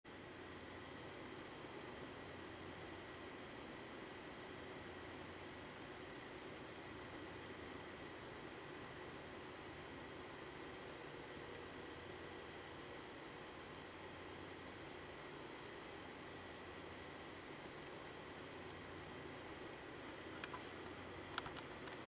Coupling Up To Annie And Clarabel At Oyonix Station
Coupling up to my coaches Oyonix Station.